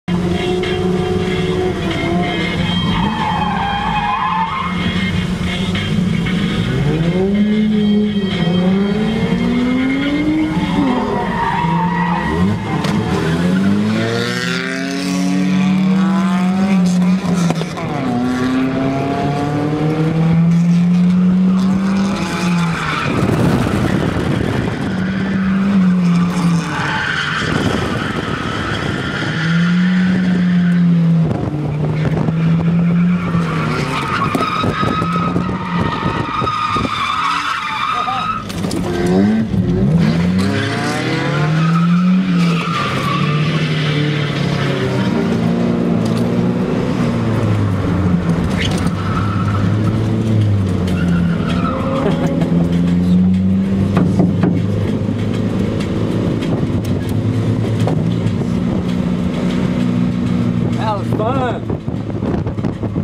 Irwindale Sideways Sundays TRUCK TANDEM DRIFT RUN (in car camera)
2 truck enter, 1 truck leave.  We were the only 2 trucks drifting the event.
Pesja in the Hardbody was driving better than I was :).
This is a quick upload of one of the runs that we got at Irwindale.
...ended in a spin but was fun anyways! :)
Nissan Hardbody
Nissan Frontier